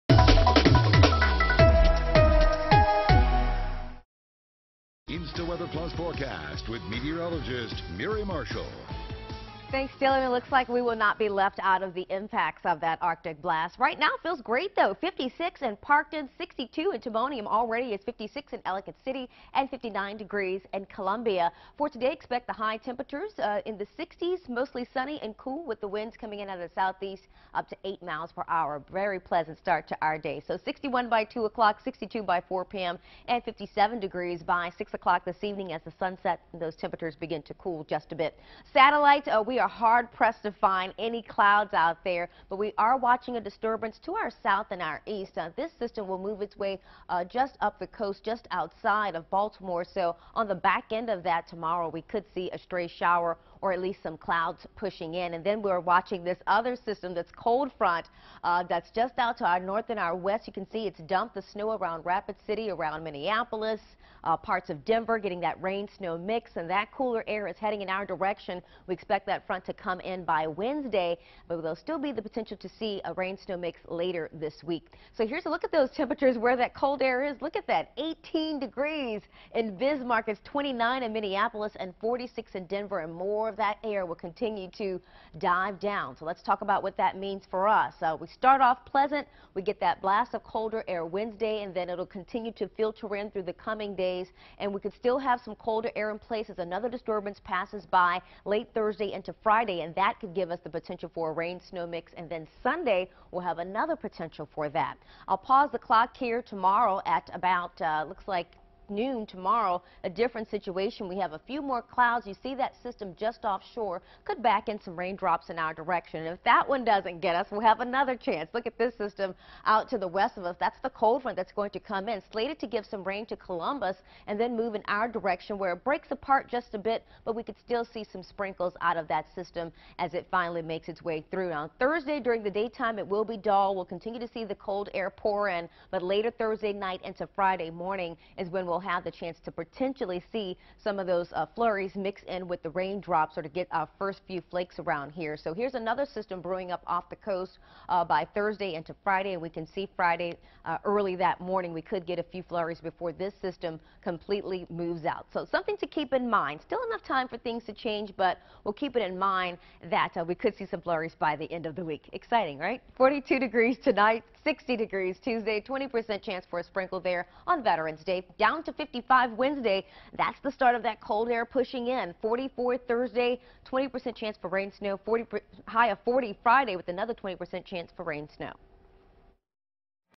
Mostly sunny, cool conditions today
During the Monday afternoon weather forecast, Miri says we can expect mostly sunny and cool conditions in the region.